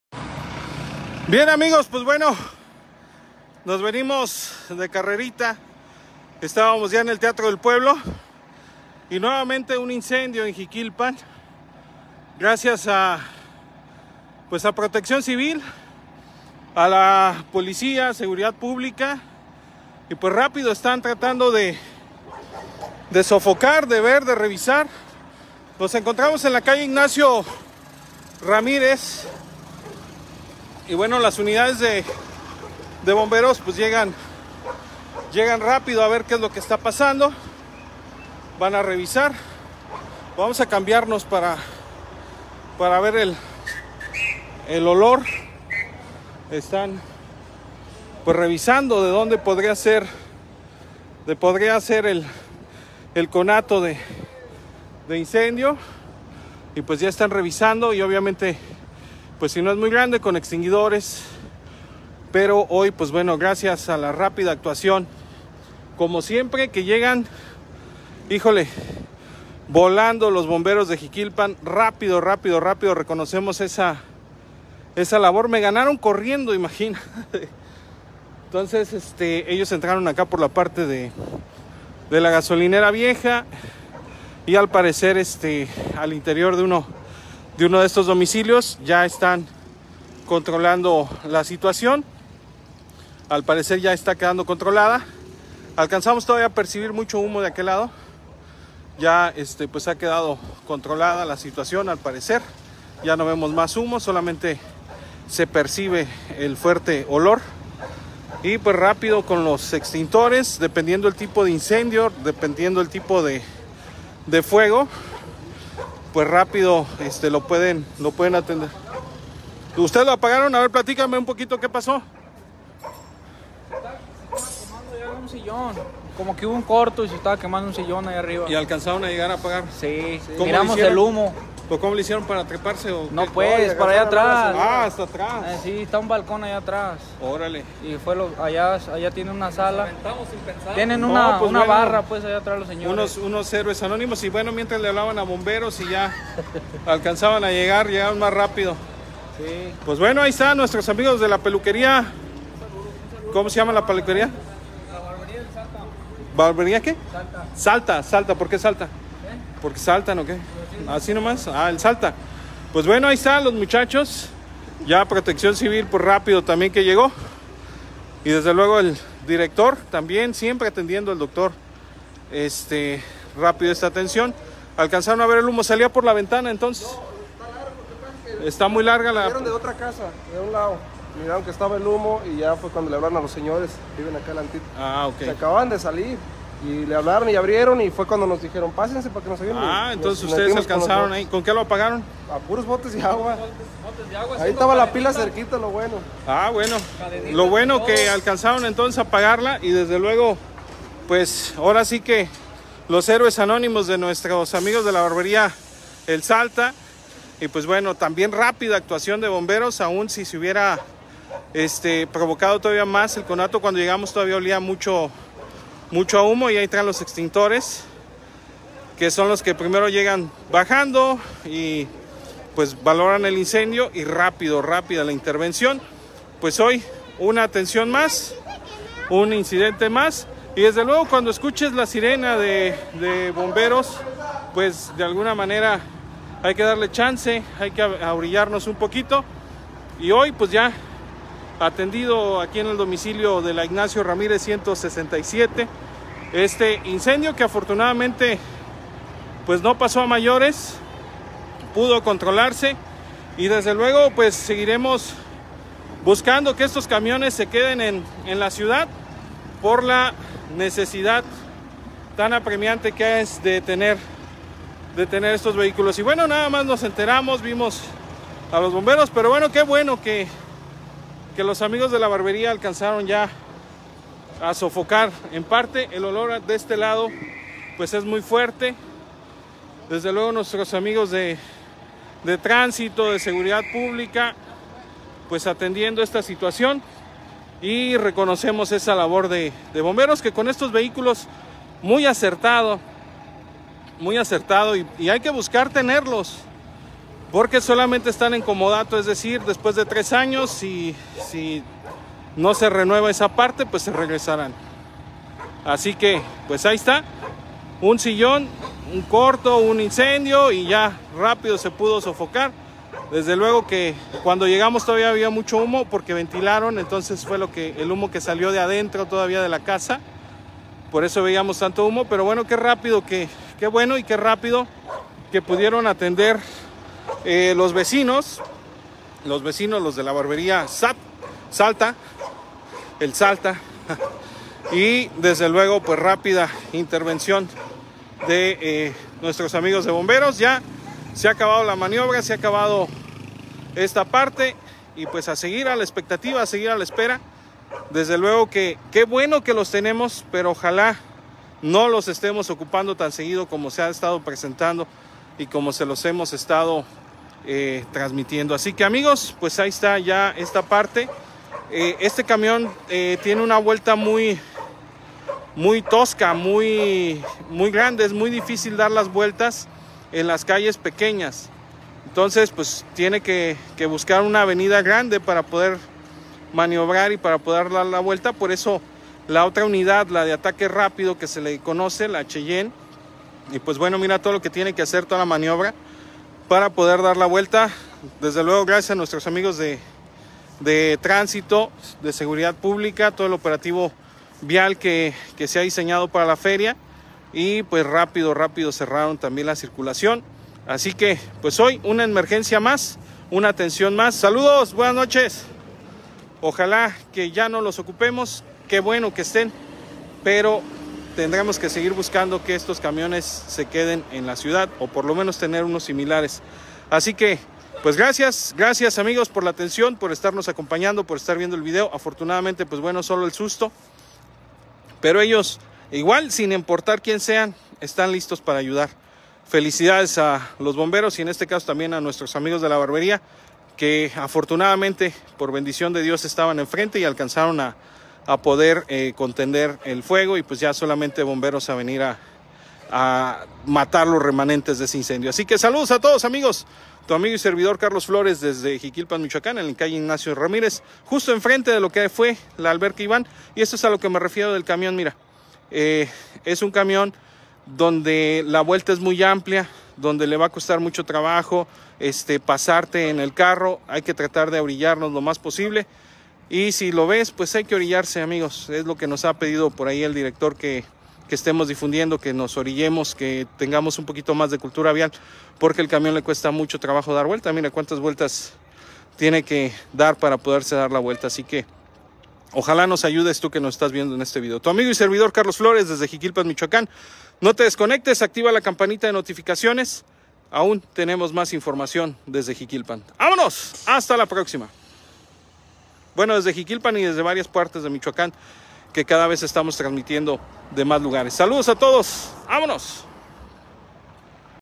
Incendio en Jiquilpan sofocado por los amigos de la Barbería
Incendio en Jiquilpan sofocado por los amigos de la Barbería y atendido por Proteccion Civil de Jiquilpan quedó bajo control